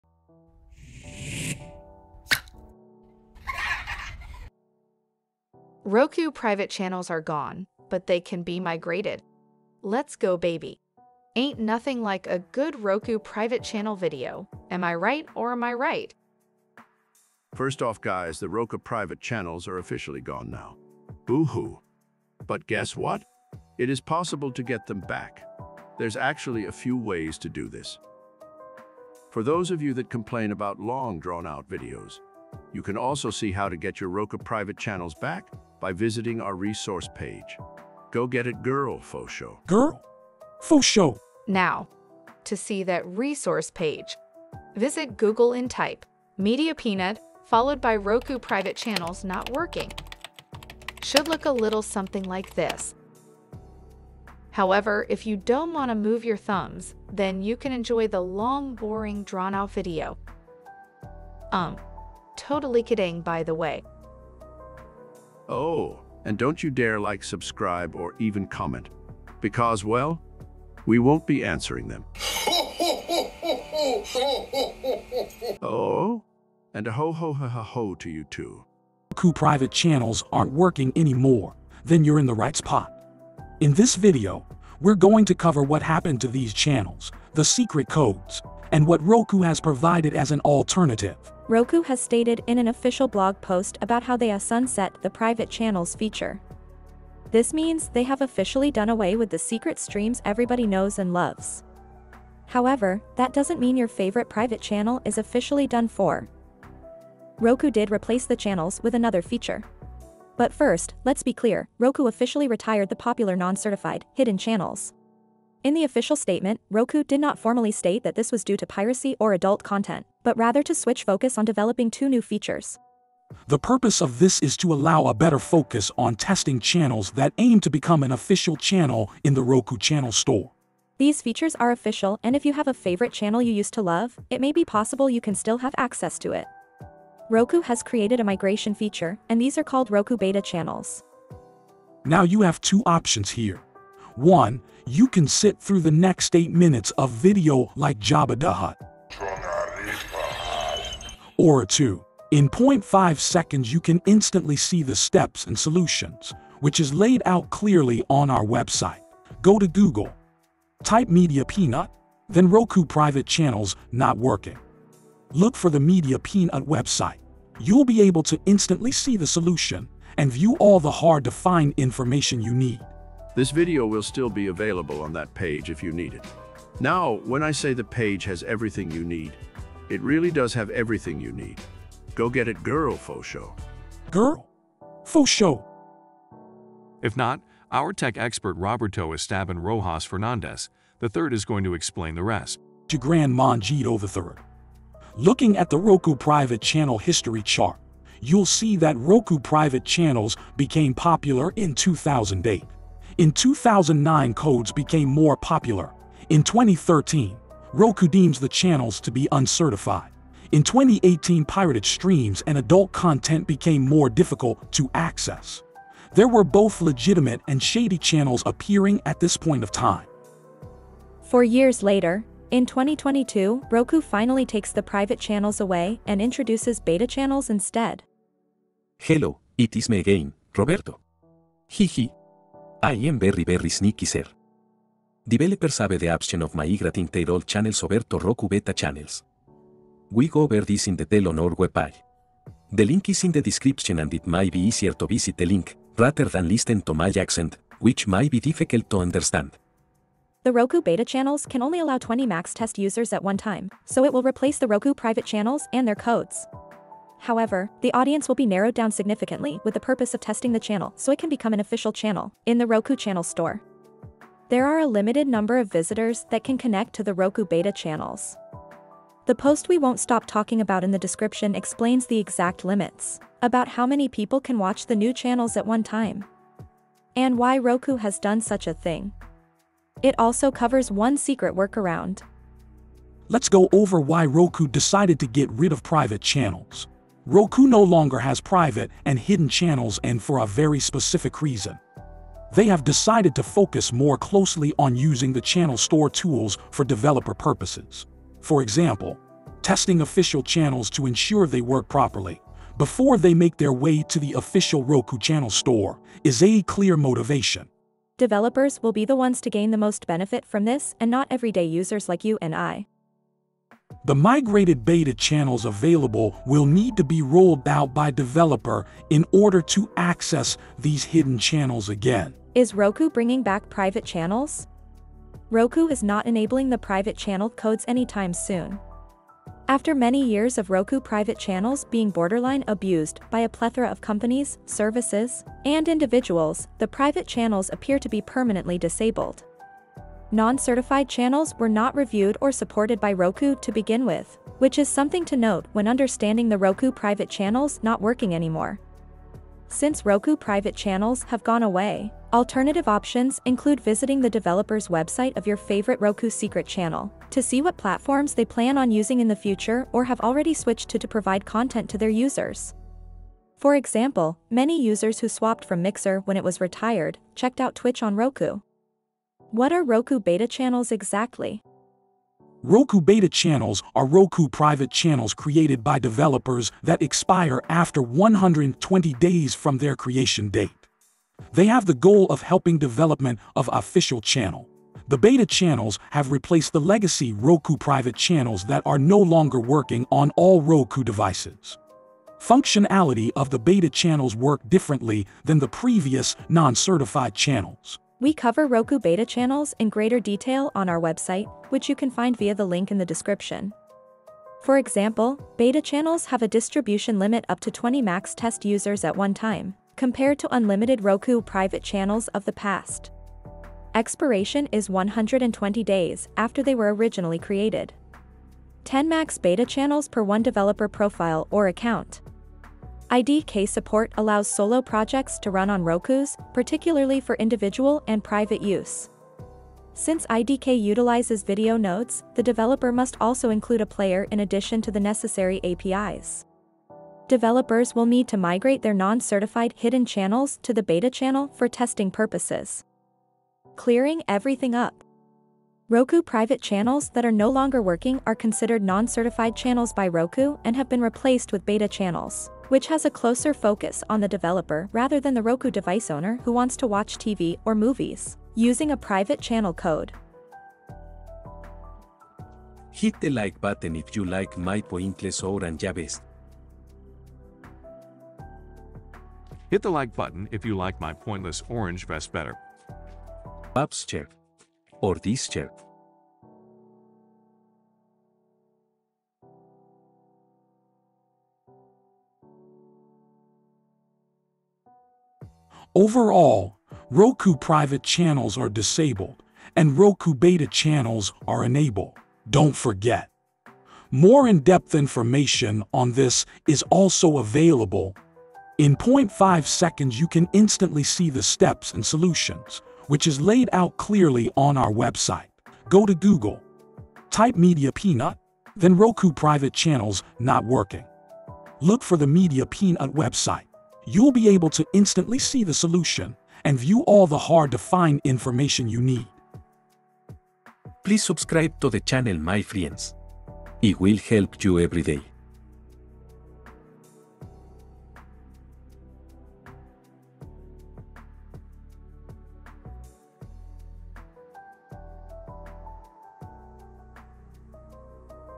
Roku Private Channels Not Working Anymore and how to fix it with beta channels
It may come to a shock for many when they find out their favorite Roku Private channels are not working anymore, but can this be fixed? Feb 23, 2022 is the date Roku stated that it will disable all uncertified channels.

Roku users are left wondering which Roku device the hidden channels won’t be compatible with anymore.

00:00 Intro
0:23 Are Roku secret channels gone?
0:43 More in depth information on Roku private channels
1:04 Santa educates on Roku
1:39 Can you get Roku private channels back?
2:10 New Features in place of Roku secret channels
2:42 Two important options
3:27 What our tech experts say
4:48 Roku Beta Channels instead of Private now
6:26 How to access Roku private channels again
8:00 Quick Summary of everything
9:06 Conclusion

What happened to the Private channel codes?
Roku has decided to replace private, non-certified channels with two new features that will allow the migration of private channels to “Roku beta channel”; a feature that limits access to 20 users at a time, however this could change.

These are channels that have a limited number of users with the purpose of testing a channel, so it can ultimately become official on the Roku channel store.